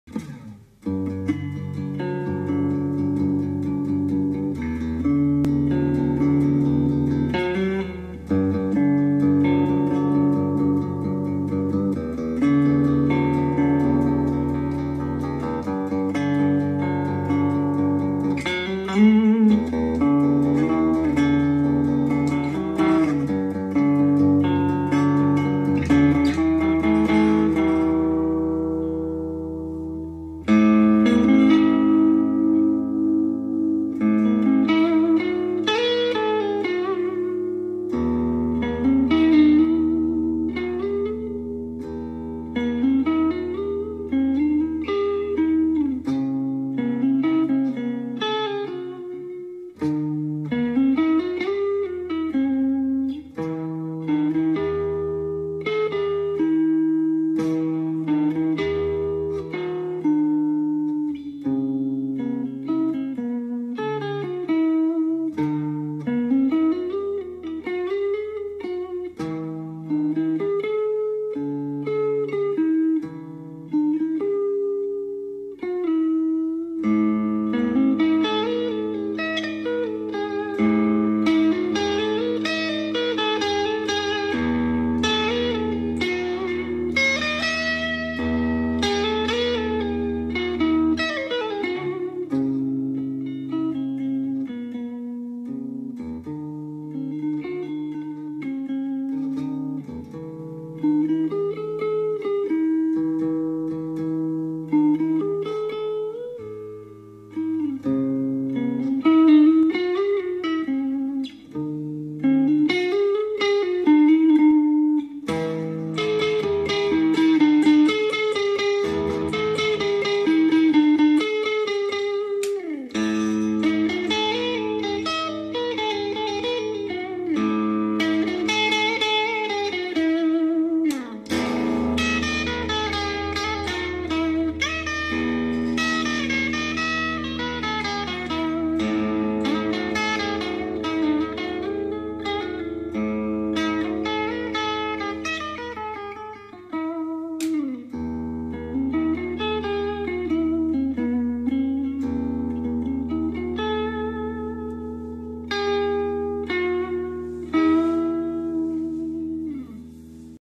WILKINSON MWCHB pickups ✅
Nothing serious here, just jamming. I thought it would be okay to upload even if if its crappy camera audio. No talking
But i think it still shows the good musical sound these pickupus can deliver.
The guitar is an old SC-400 which has been getting wilkinson pickups of an sc-450.